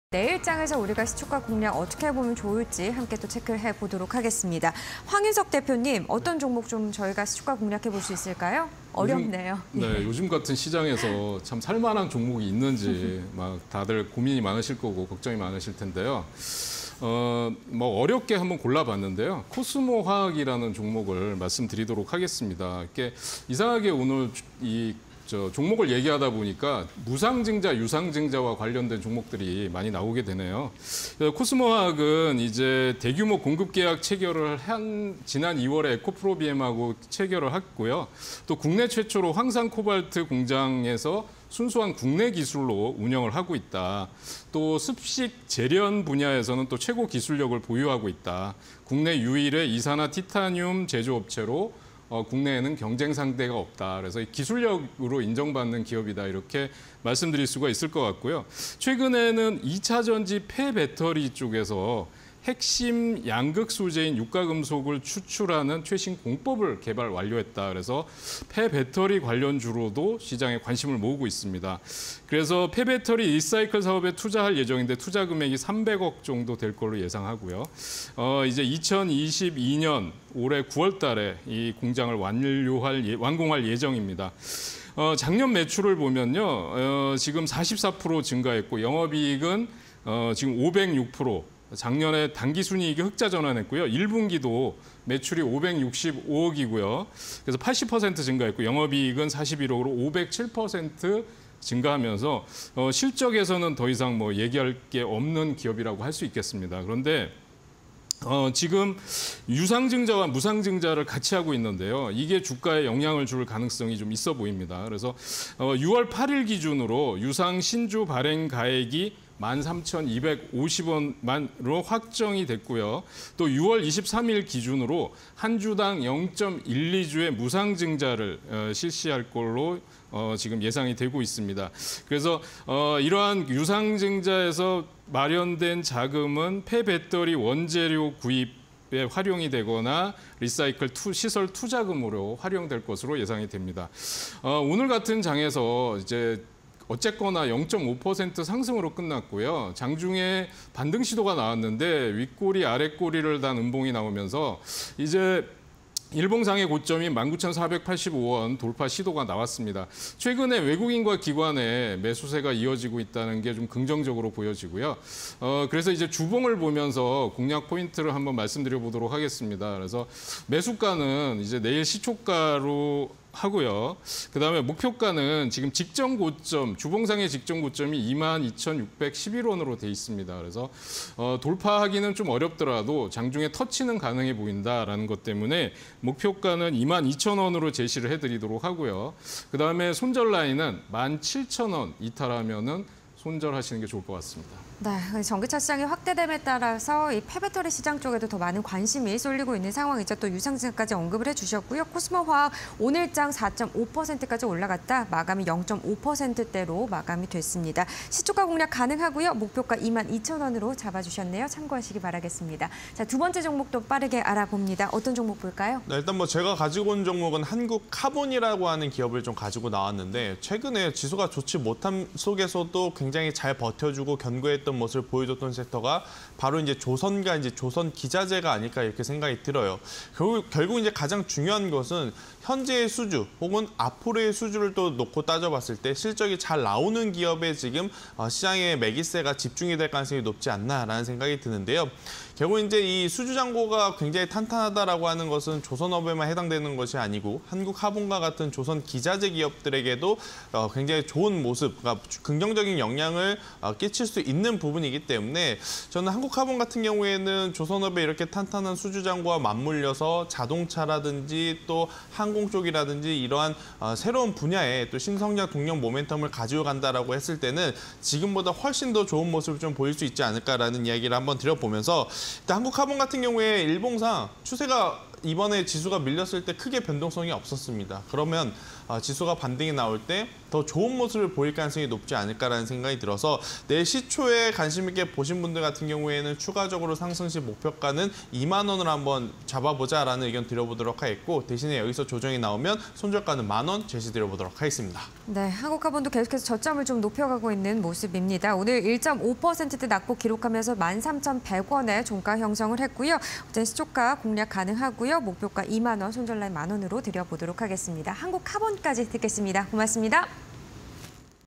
코스모화학(005420), 한국카본(017960)_사가지 (20220620)
코스모화학(005420)
- 지난 2월, 341억 규모 황산코발트 공급계약 체결
- 폐배터리에서 유가금속 추출 공법 특허출원
- 지난해 영업이익 약 506% 증가

한국카본(017960)
- LNG 운반선용 단열판넬ㆍ탄소섬유 업체
- 확실한 수주잔고ㆍ선가 상승 반영 수혜
- 신규 고객 수주 확대… 외형 성장 기대